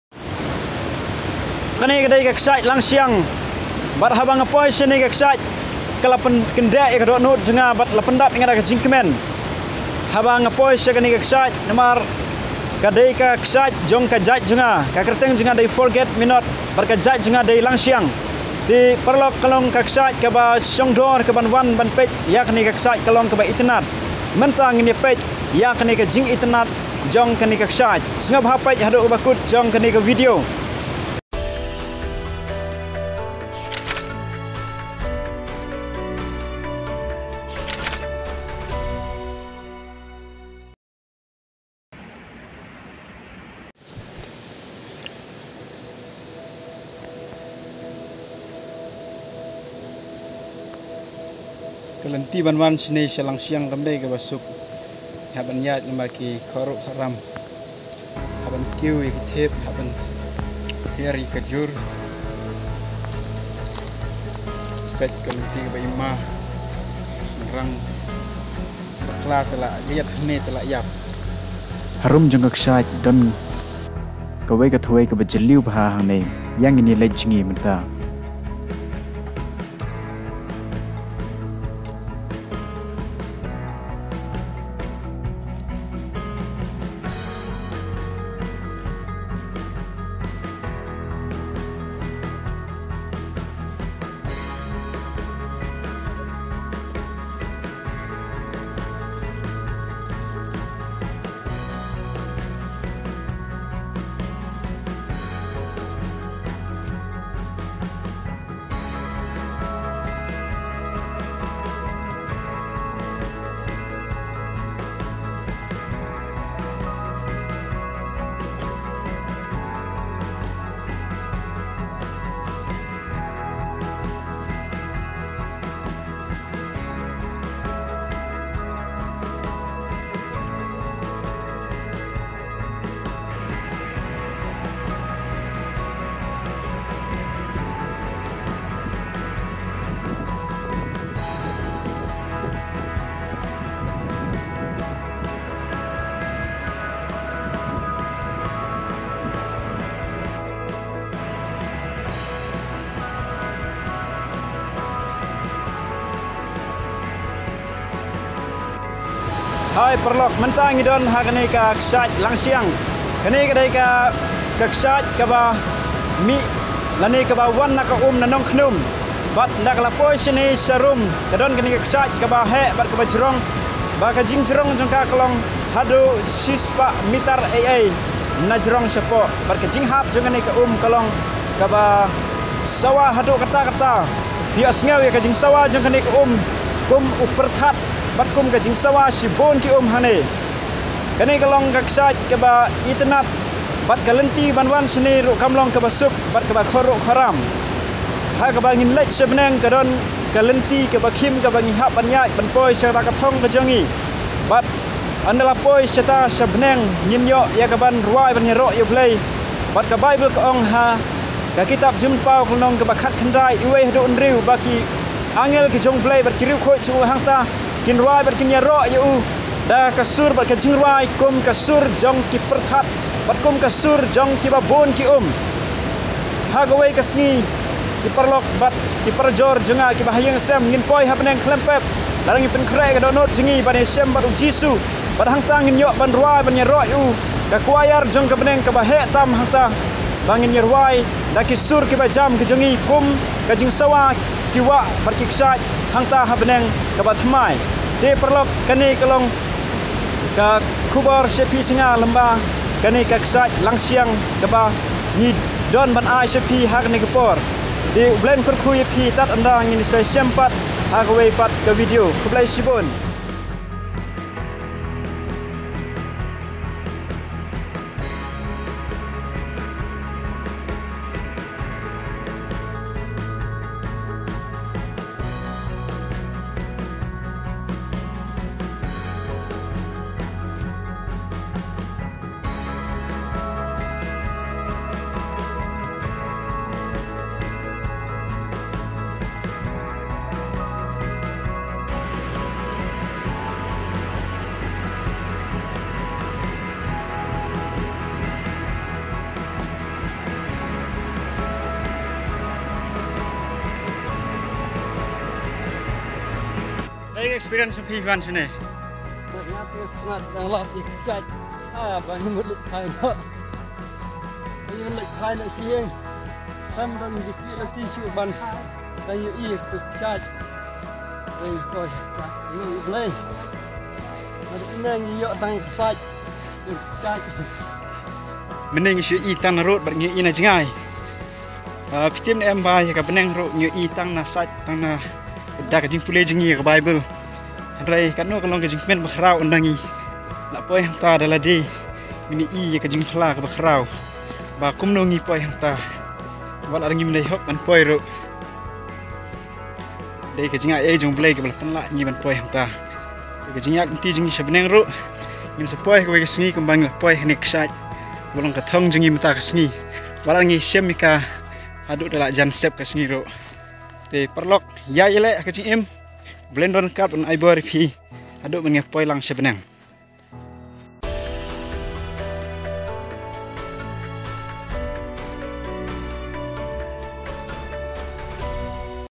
Kshaid Langshiang | Langshiang Waterfall (Sangriang), West Khasi Hills.
Ha kane ka video ngin explore ia kawei ka Kshaid kaba itynnad bha kaba don hajan ka shnong Sangriang, West Khasi Hills.

Khublei shibun.